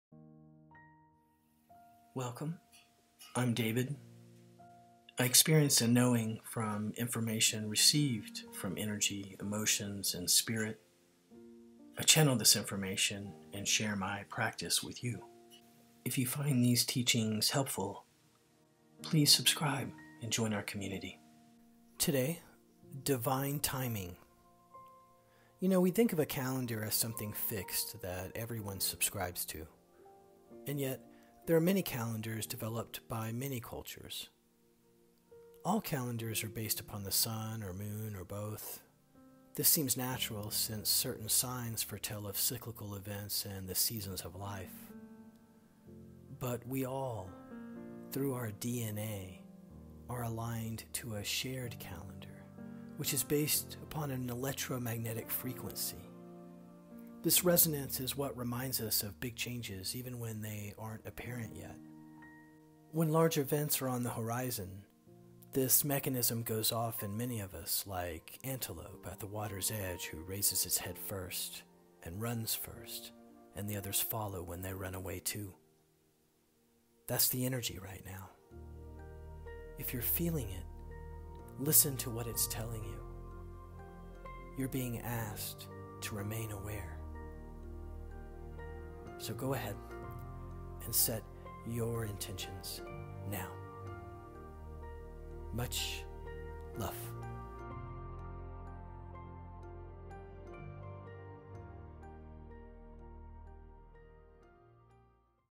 We have a shared calendar of events
Today, divine timing.

We think of a calendar as something fixed that everyone subscribes to. And yet, there are many calendars developed by many cultures. All calendars are based upon the Sun or Moon or both. This seems natural since certain signs foretell of cyclical events and the seasons of life. But we all, through our DNA, are aligned to a shared calendar... which is based upon an electro-magnetic frequency. This resonance is what reminds us of big changes even when they aren’t apparent yet. When large events are on the horizon, this mechanism goes off in many of us, like antelope at the water’s edge who raises its head first ,and runs first, and the others follow when they run away too. That’s the energy right now. If you’re feeling it... listen to what it’s telling you. You’re being asked to remain aware. Go ahead and set your intentions now. Much love.